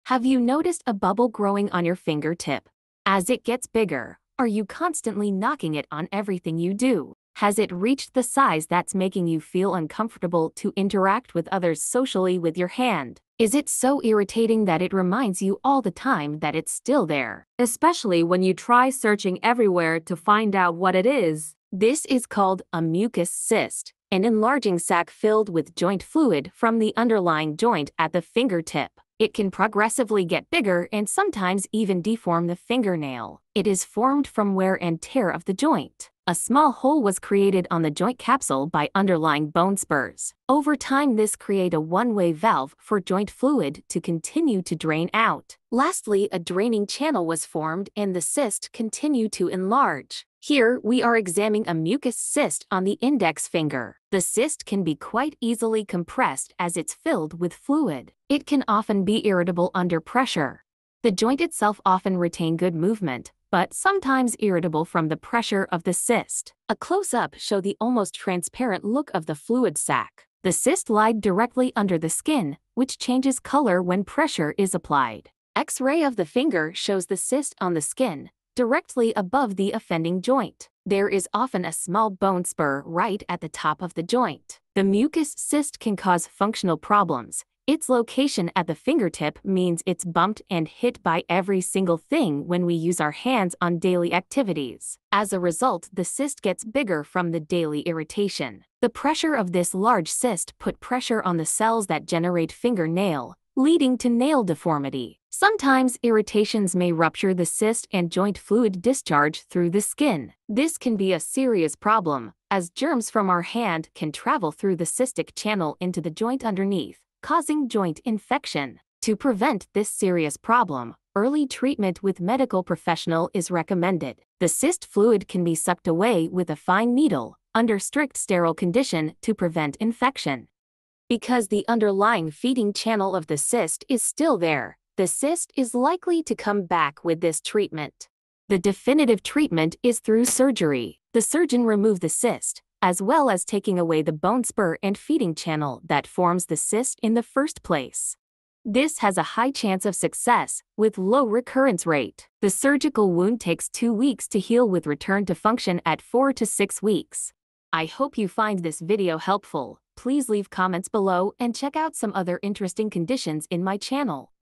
Finger Mucous Cyst : Learn to treat in 3 minutes
All you need to know about finger mucous cyst in three minutes

Finger tip protective silicon sleeve